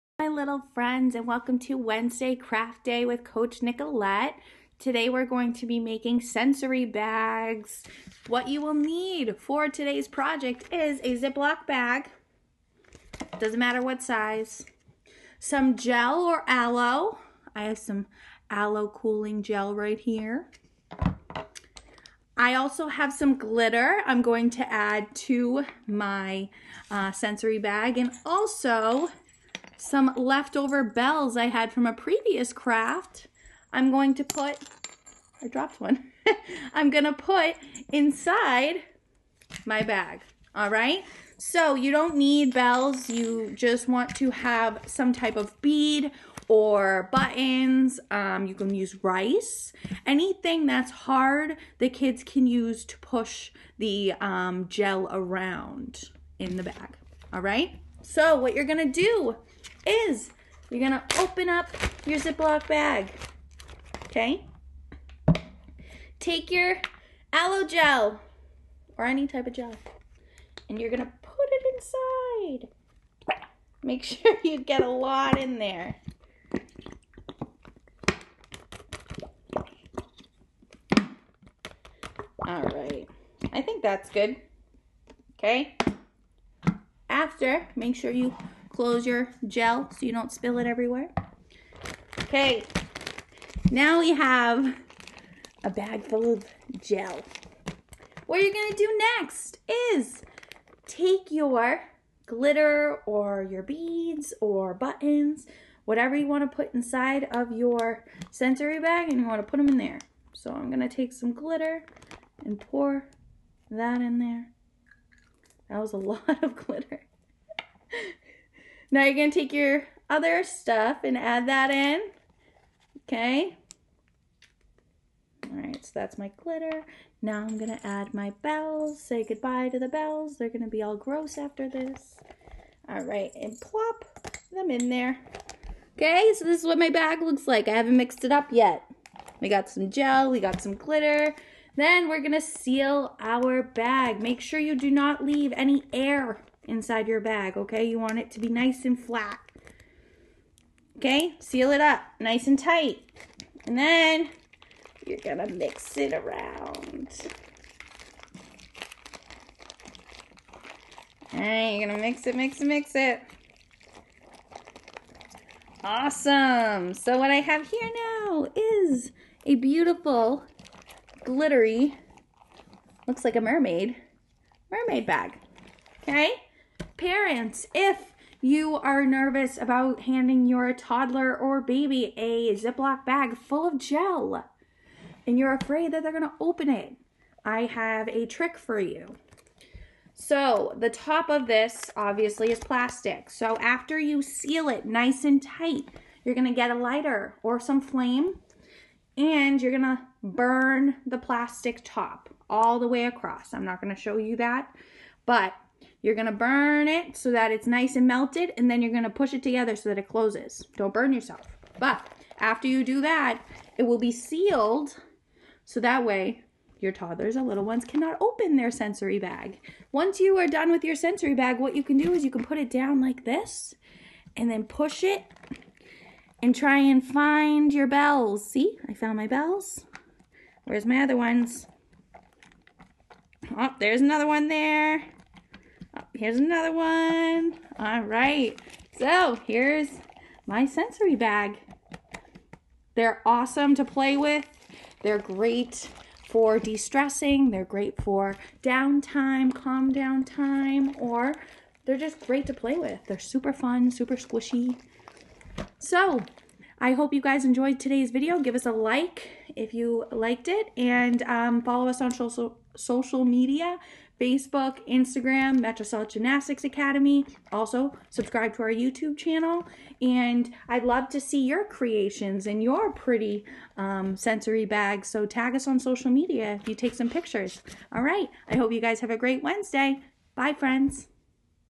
Craft Wednesday ~ Make Your Own Sensory Bag!
Engage your kiddo's senses with today's craft! It is make your own sensory bag day.

Supplies:
Ziploc bag
Gel or Aloe
Glitter
Bells or other objects to put inside the bag